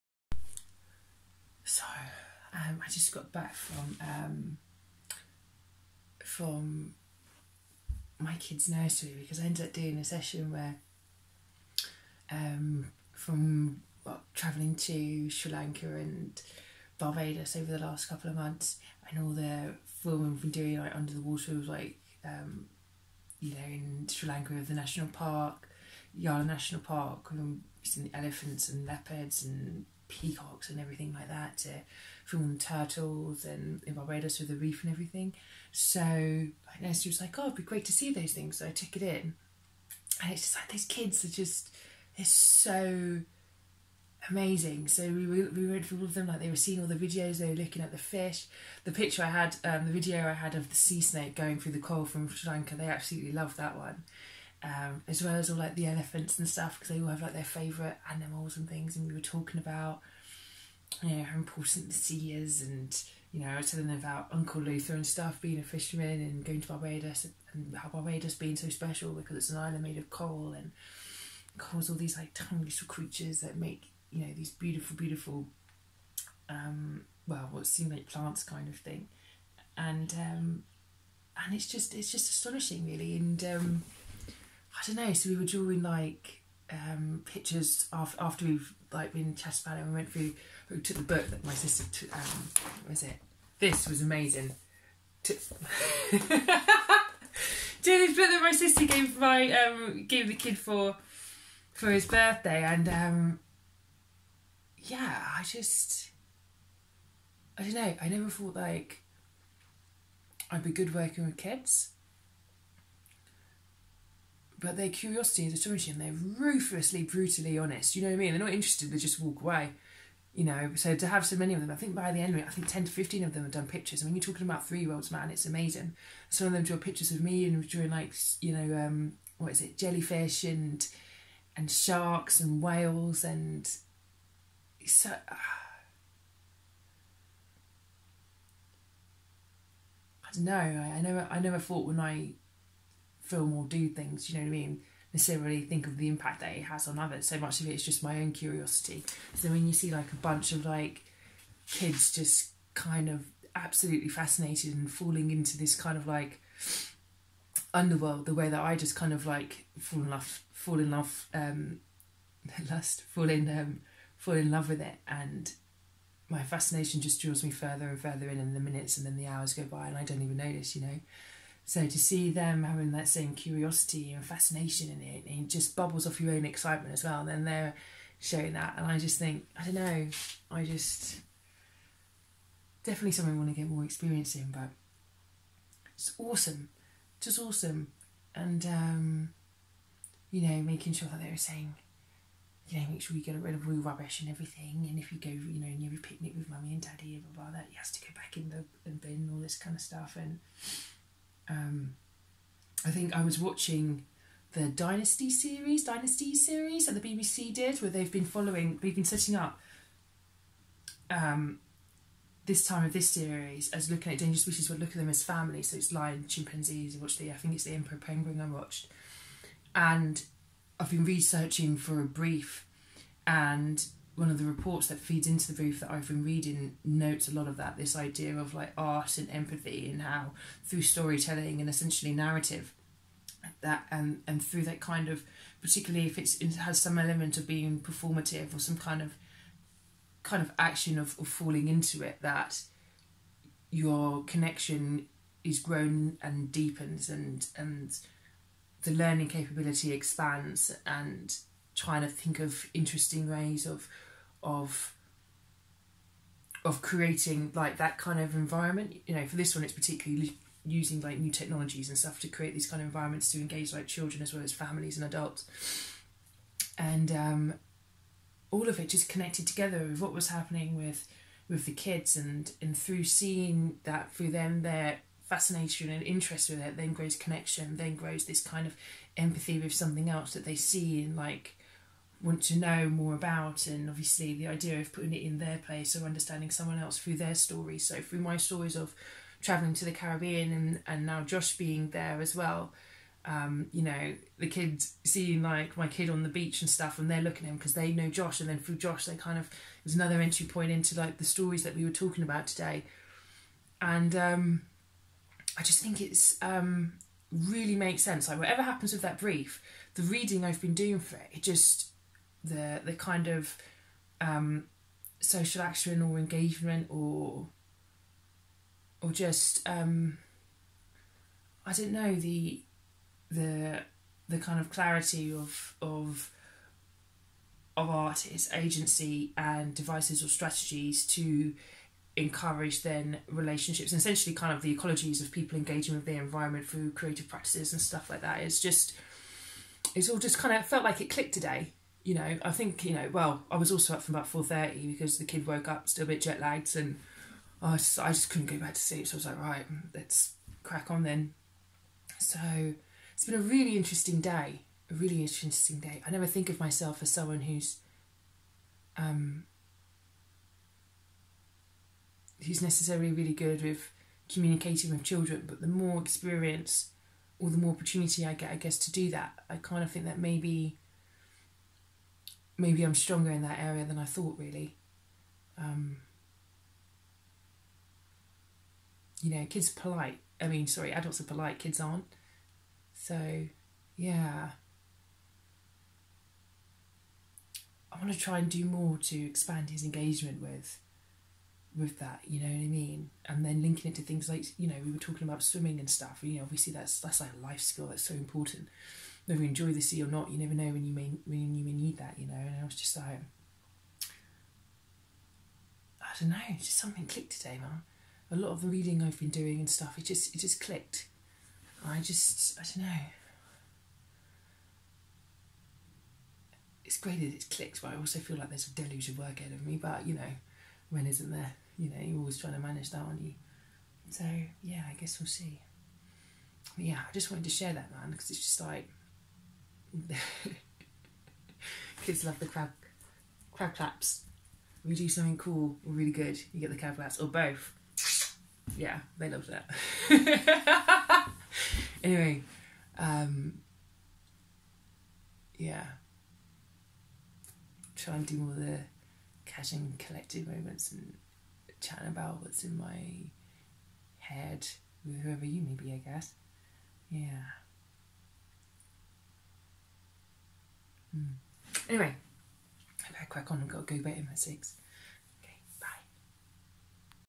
CAHJ & COLLECTED #5 - Nursery session 'Under the Sea'.
After travelling to Sri Lanka and Barbados last couple months, I took all of the videos and pictures to share with the kids and his friends about the creatures we had seen particularly under the sea.